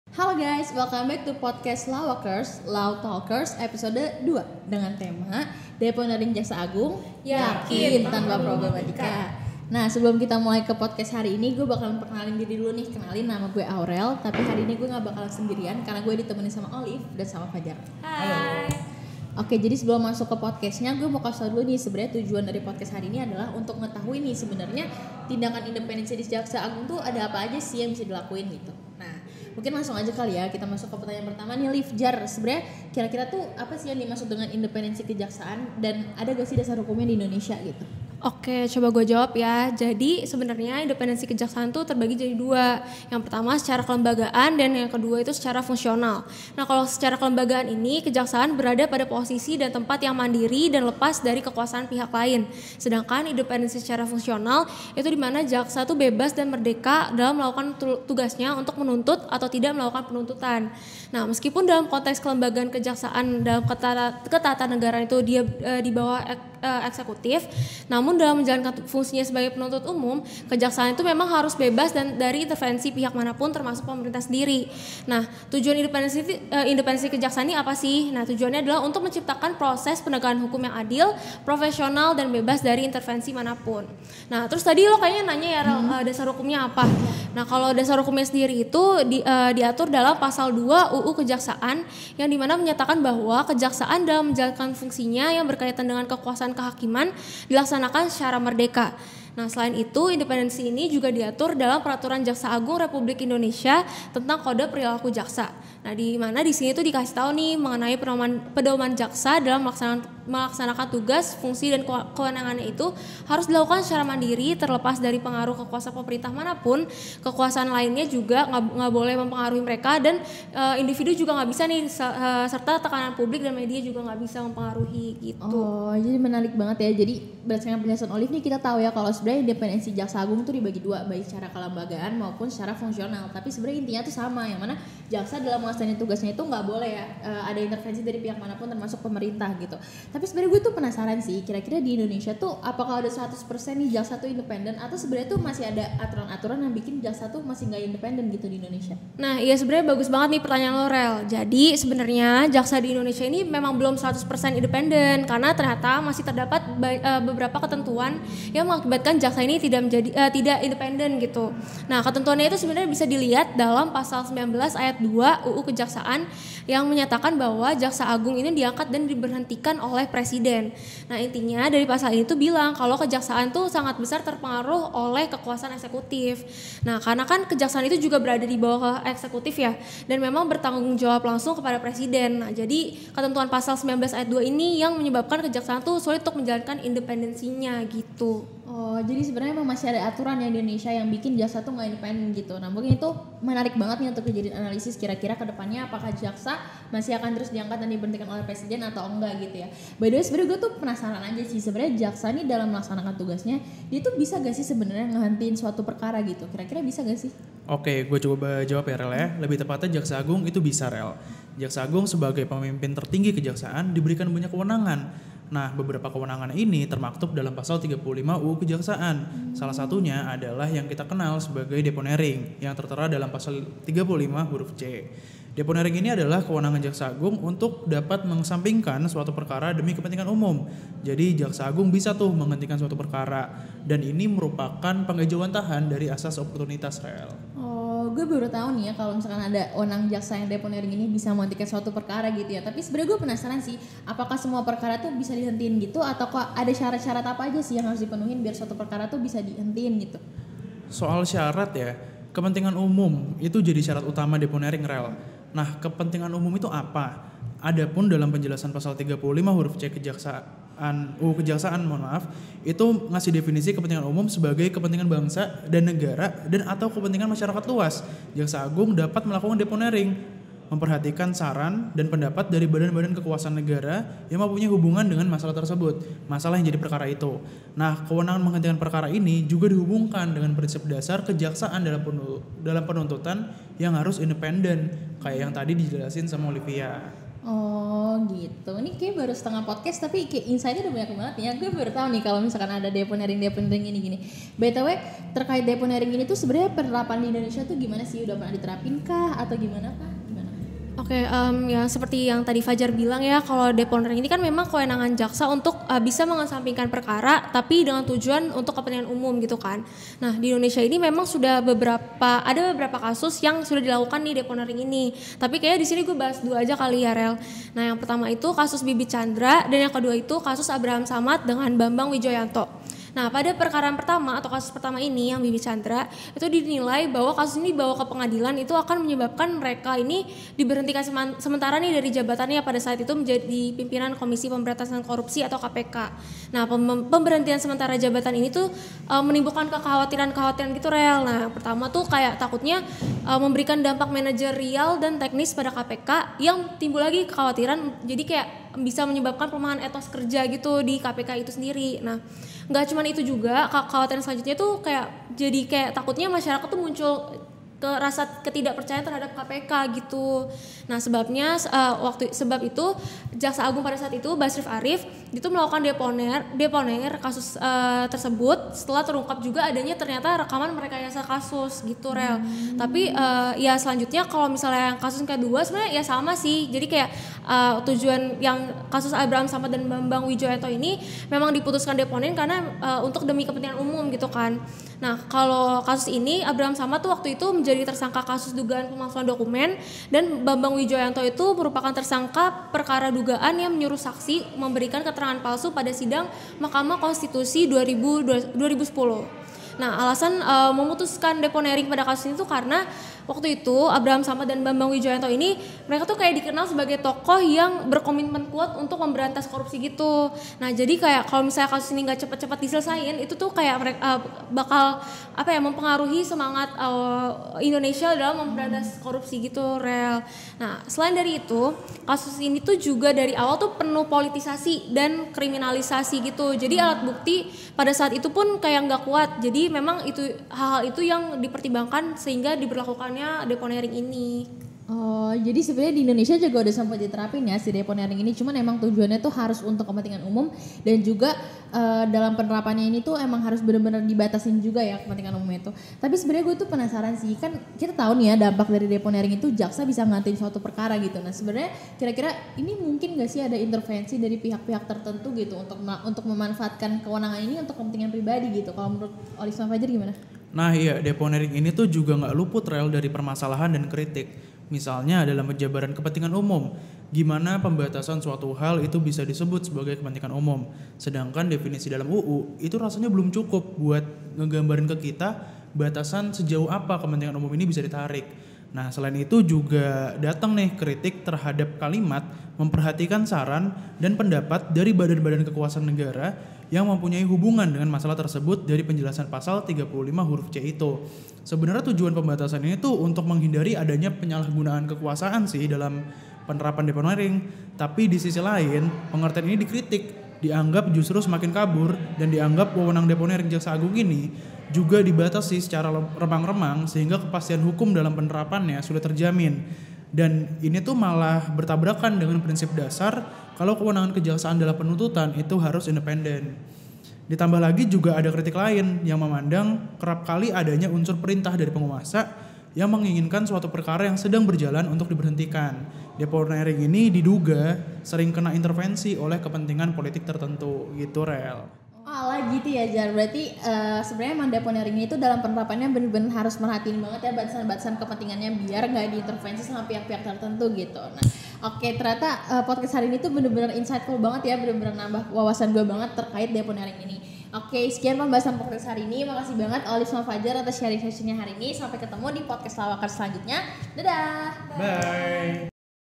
LAWAKERS! Law Talkers Ep. 2: Deponering Jaksa Agung, Yakin Tanpa Problematika?
Pada Episode #2 Podcast ini, Fajar, Olivia, dan Aurel akan membahas dan mengupas lebih dalam mengenai salah satu kewenangan yang dimiliki oleh jaksa agung, yaitu deponering sebagaimana diatur dalam Pasal 35 huruf c UU Kejaksaan. Kewenangan Deponering ini telah dilakukan beberapa kali di Indonesia. Penasaran kan? Yuk langsung aja simak Podcast-nya!

Kelompok 5 Jaksa ETJP Reguler A:
1. Adiva Maysa Rania (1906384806)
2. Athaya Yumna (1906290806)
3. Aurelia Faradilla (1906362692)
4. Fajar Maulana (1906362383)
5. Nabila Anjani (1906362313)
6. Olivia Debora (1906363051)